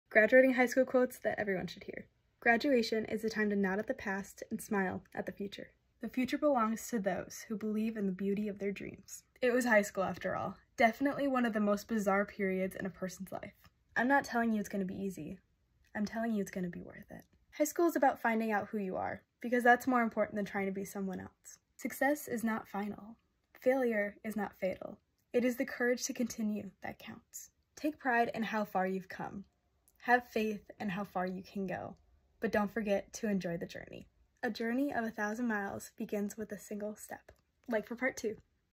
Quotes about graduating high school (from a senior)
Some quotes about graduating high school that hit different as a senior in high school...